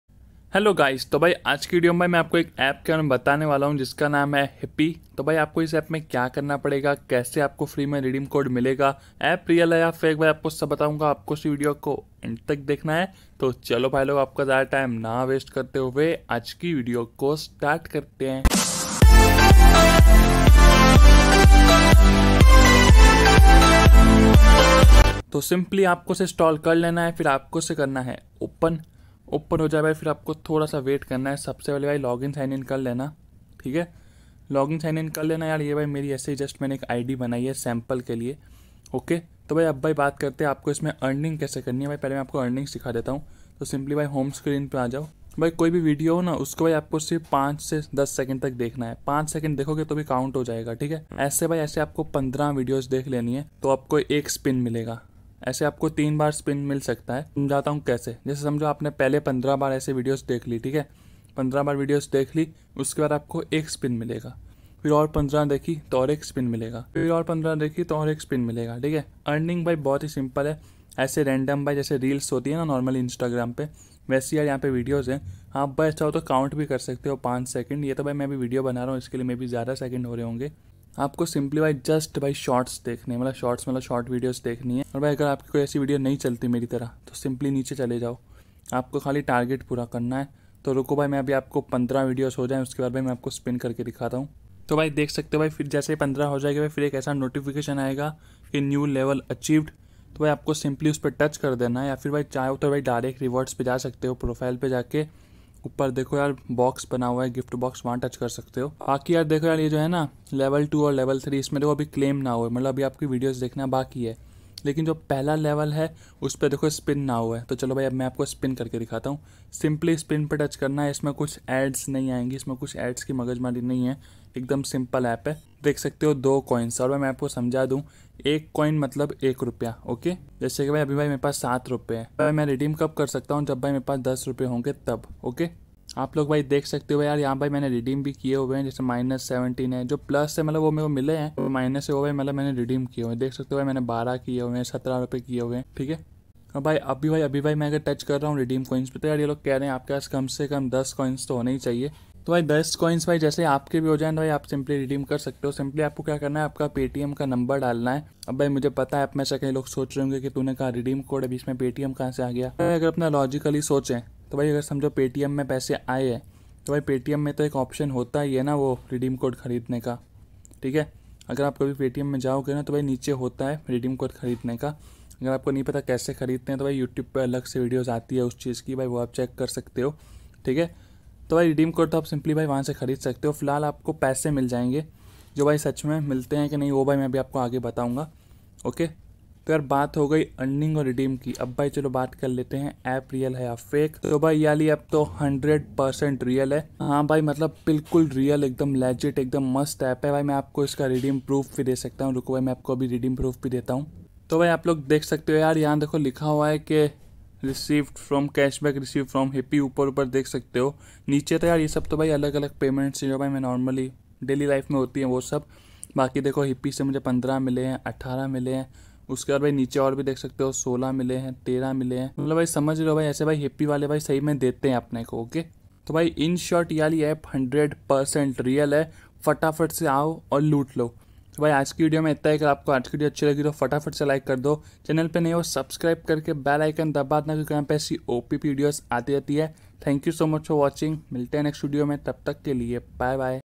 Hipi app real or fake | free redeem code | Garena free fire | Tech Lover DK
Hello viewers

In this video i am going to tell you about a app named hipi